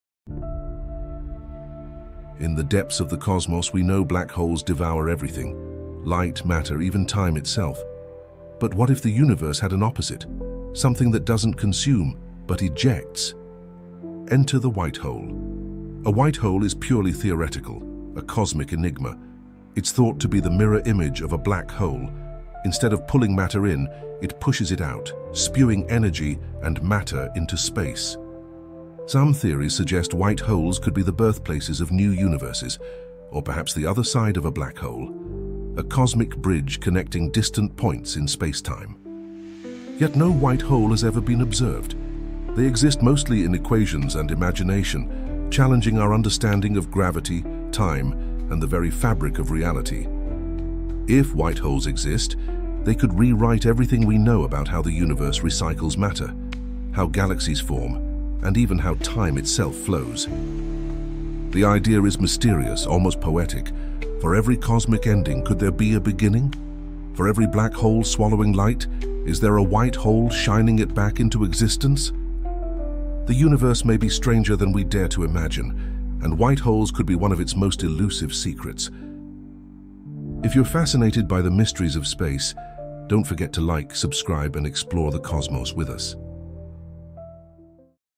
White Holes: The Universe’s Mysterious Opposite of Black Holes ✨
We all know black holes devour everything — light, matter, even time.
But what if the universe had an opposite? Something that ejects instead of consumes?

Enter the white hole — a theoretical cosmic enigma, the mirror image of a black hole. Instead of swallowing matter, it pushes it out, possibly even creating new universes.

While no white hole has ever been observed, their existence challenges everything we know about gravity, spacetime, and the flow of time.

Could white holes be the universe’s way of balancing creation and destruction?